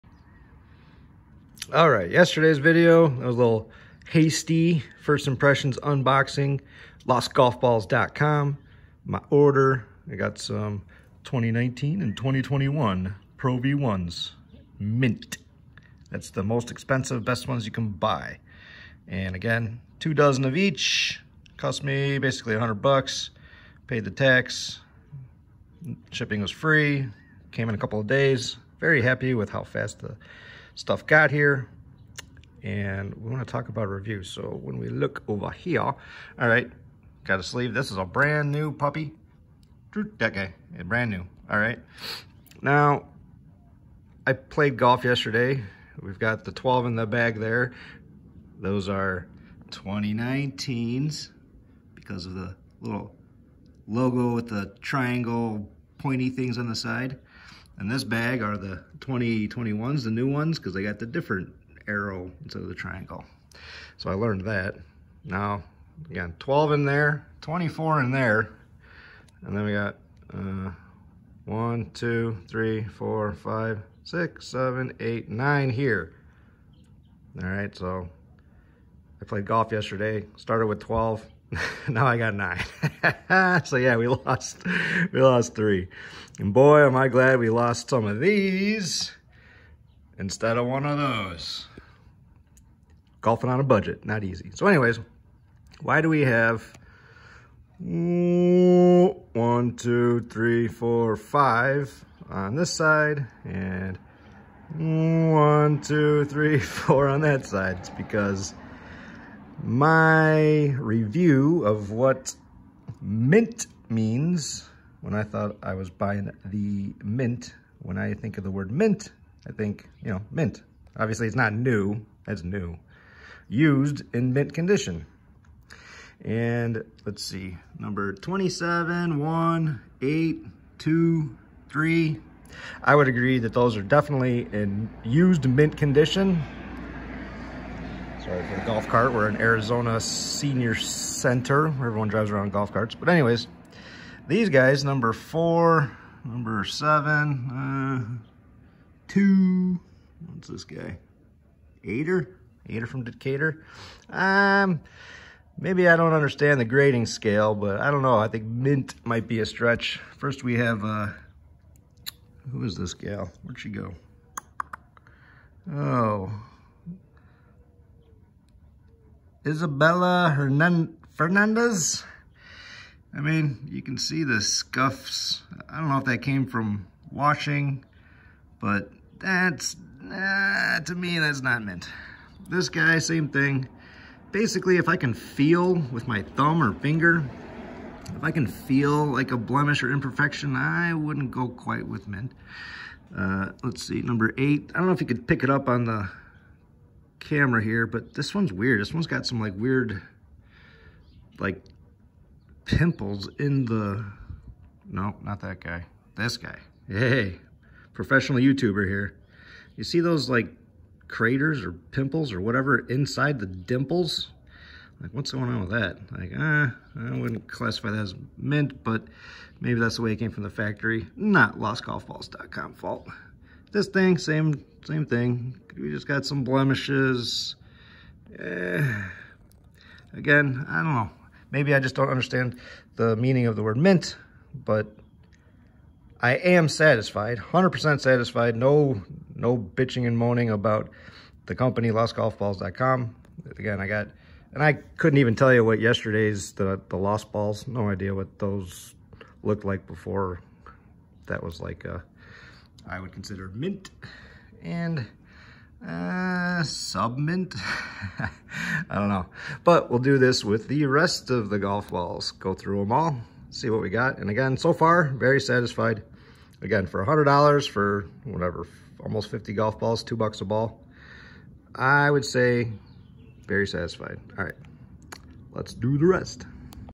LostGolfBalls.com Unboxing (Part 2 of 4)
2nd video of the PRO-V1's I bough from an online store.
Mint vs. Maybe Mint?
I learned how to tell the difference between 2019 & 2021 Titleist PRO-V1's